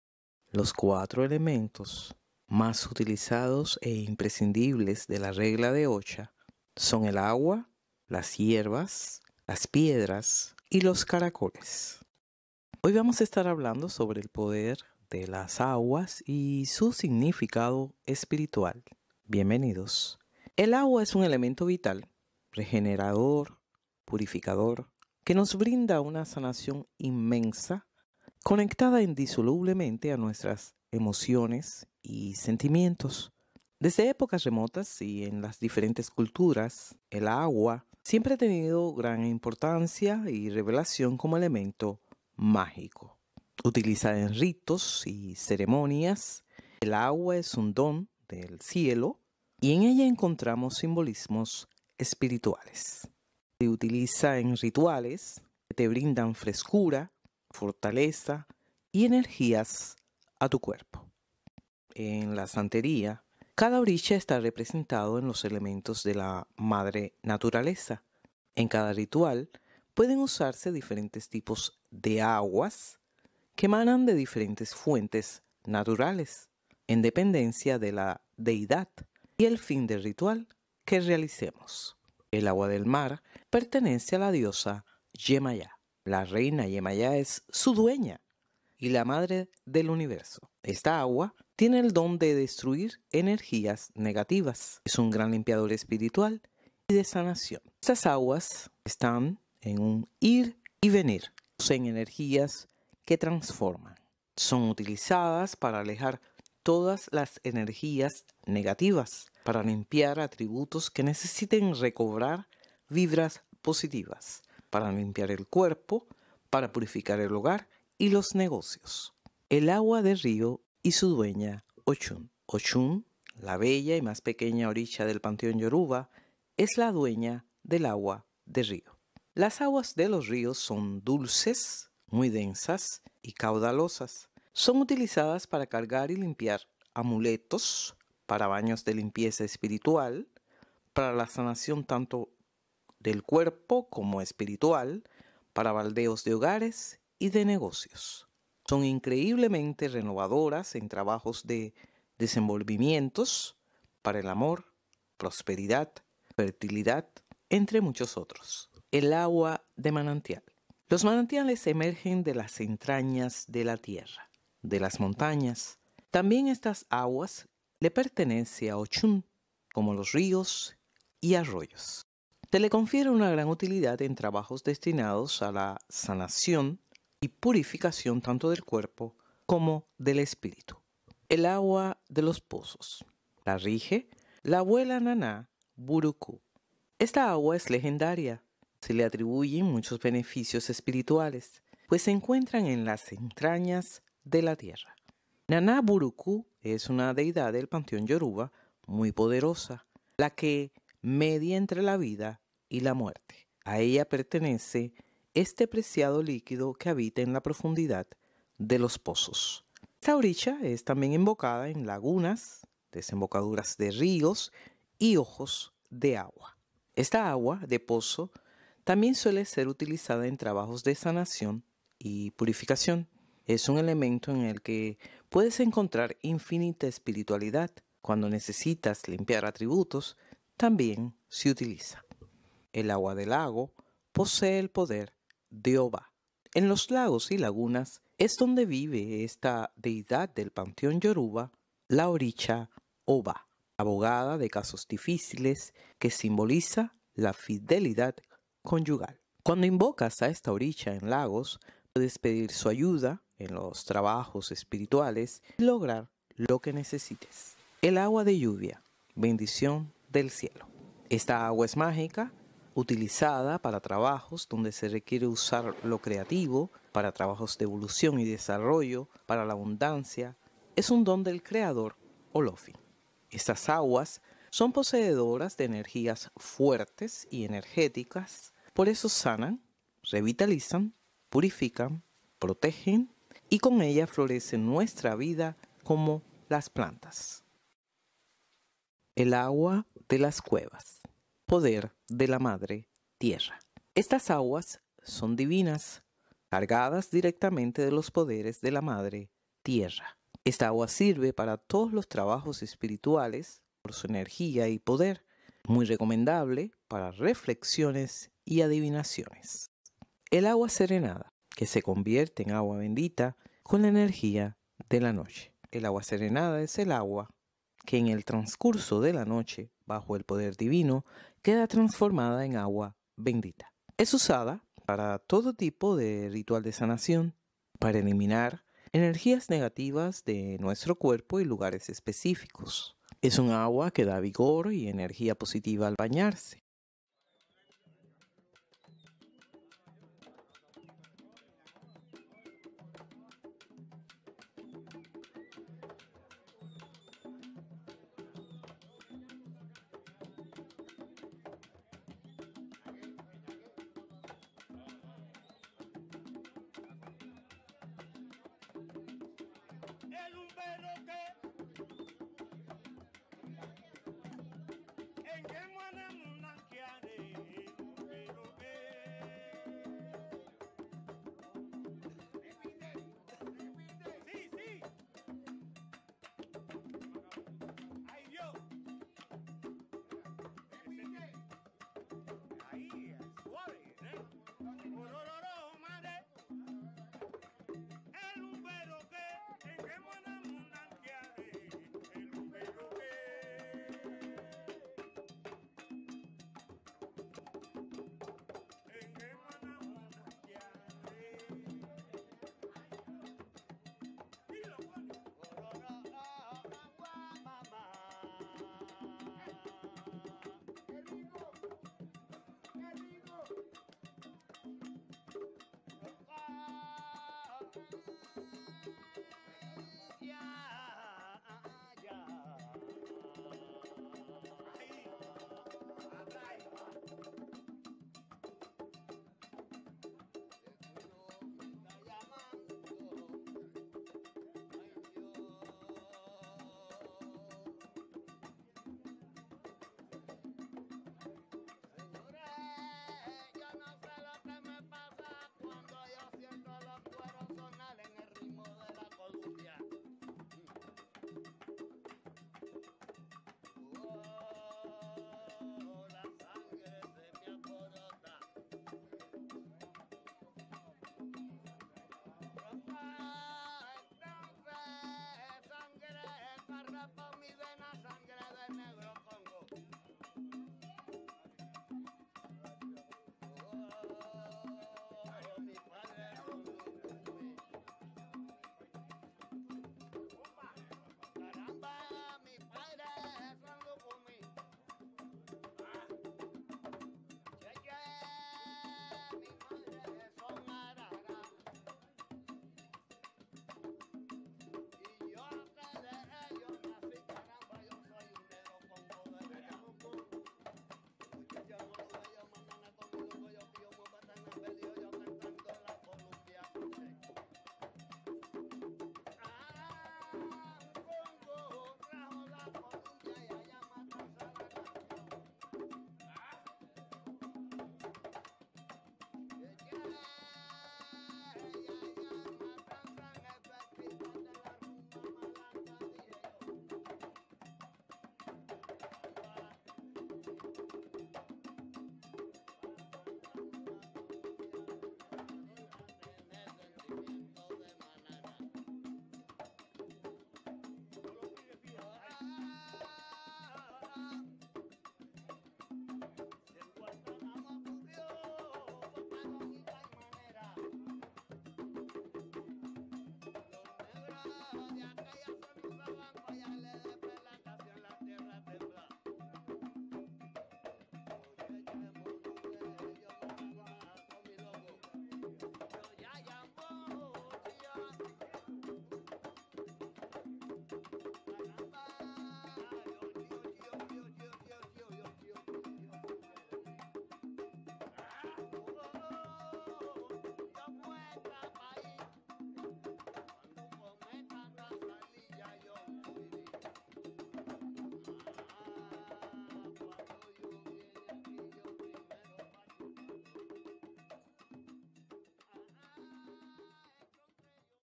Orishas Y Aguas
Te comparto este video sobre el poder y los beneficios espirituales de los diferentes tipos de aguas; conectados a los Orishas de la Regla Osha Ifá.

1:25 Agua de Mar - Yemayá
2:00 Agua de Río - Oshún
2:39 Agua de Manantiales - Oshún
3:01 Agua de Pozo - Nana Burukú
3:53 Agua de Lagos y Lagunas - Obbá
4:17 Agua de Lluvia
4:50 Agua de Las Cuevas
5:10 Agua Serenada
5:52 Ritual o Baño con agua de Yemaya